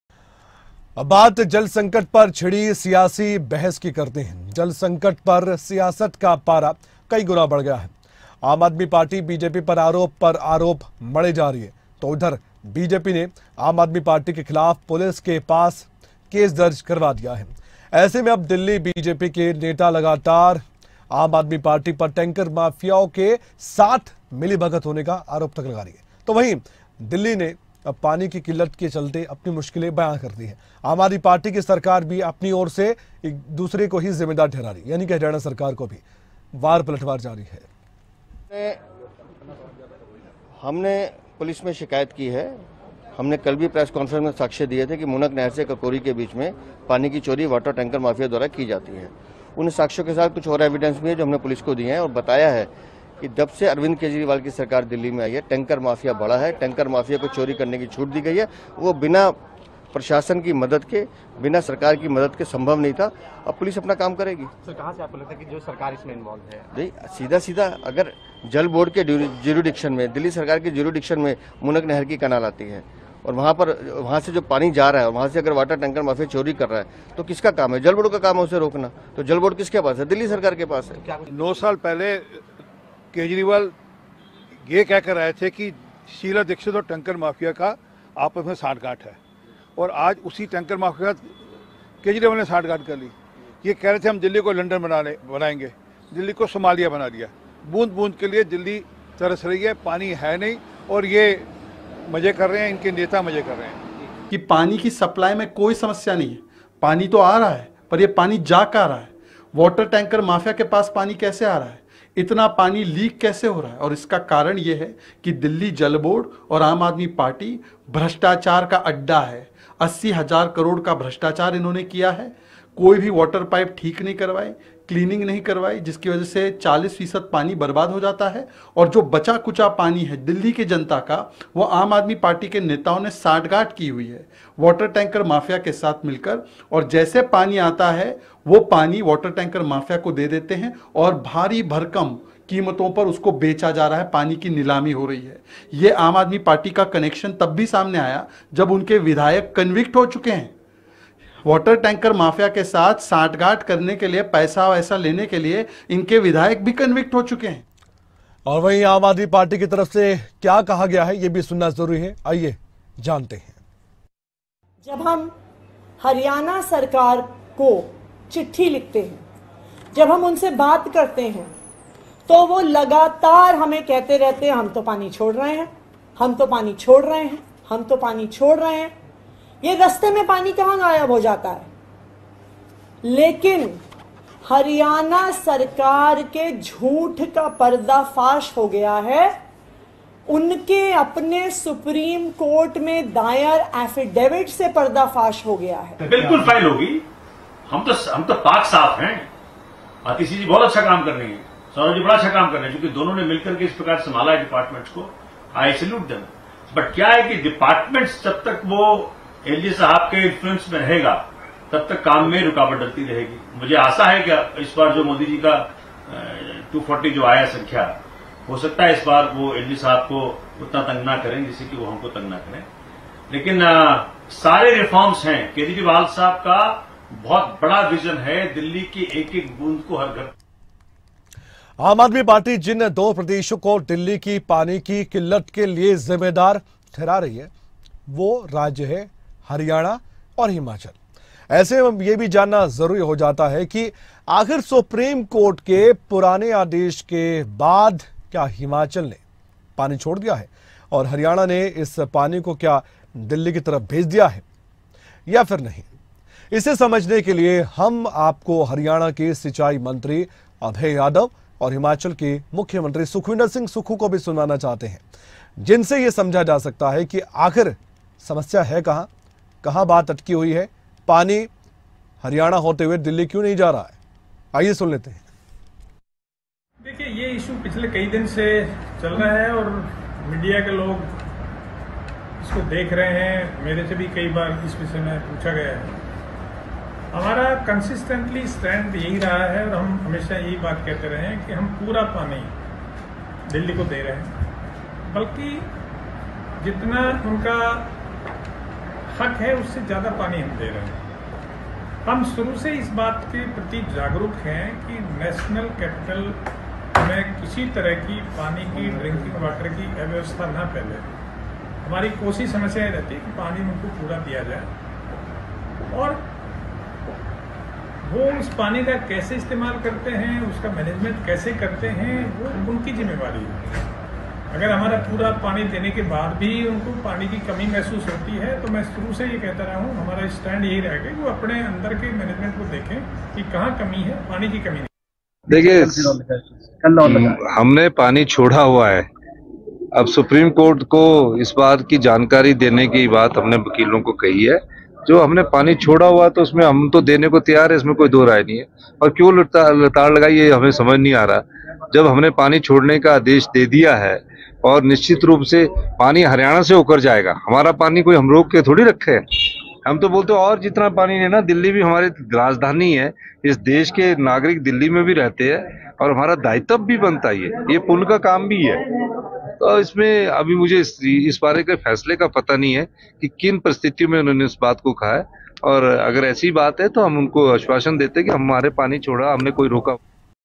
जल पर जनता परेशान..सियासत की फुल रफ्तार, दिल्ली जल संकट पर सुनिए कौन क्या बोला? | Delhi Water Crisis
एक तो गर्मी की मार, बिना पानी के हाल- बेहाल